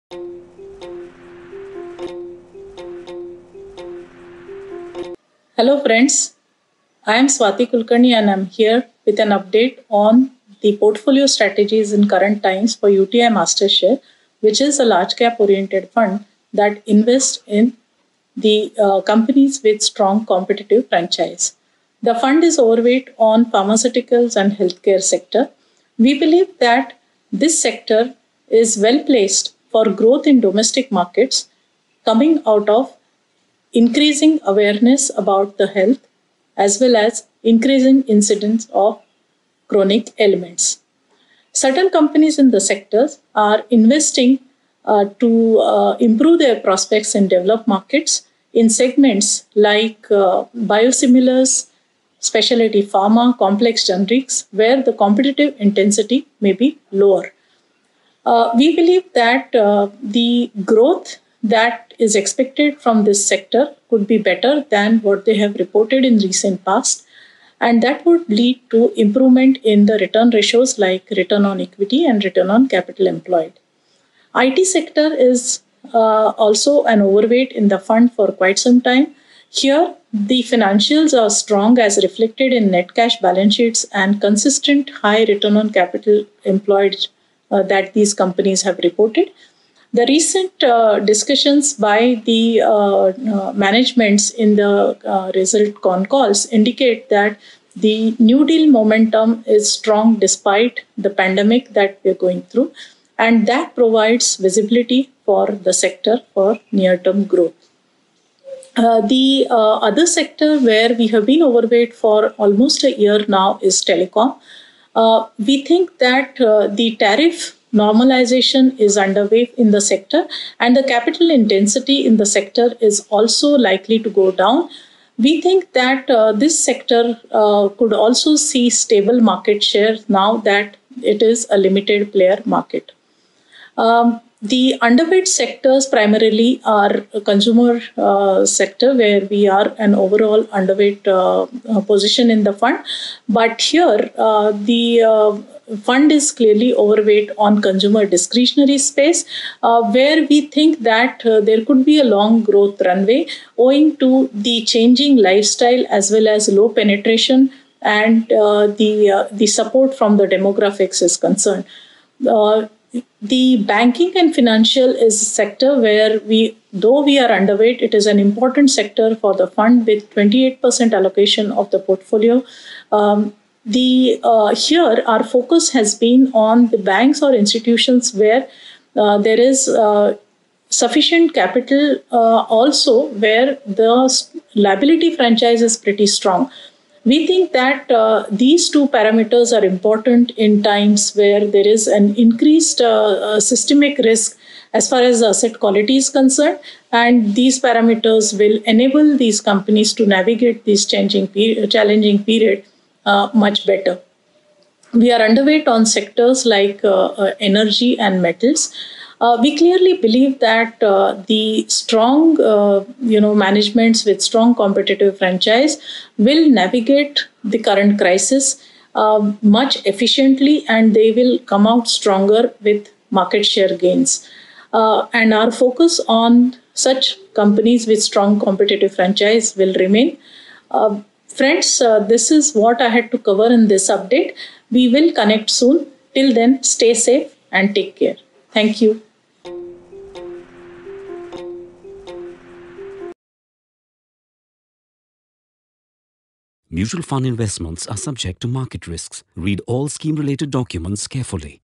UTI Connect | Portfolio Snapshot - UTI Mastershare Unit Scheme - August 2020
Watch Mrs Swati Kulkarni, Fund Manager at UTI MF, talk about the portfolio strategies in current times for UTI Mastershare Unit Scheme.

UTI Mastershare Unit Scheme is a large-cap oriented fund that invests in companies with the strong competitive franchise. Learn more about the investment, portfolio allocation across various industrial sectors, and its performance.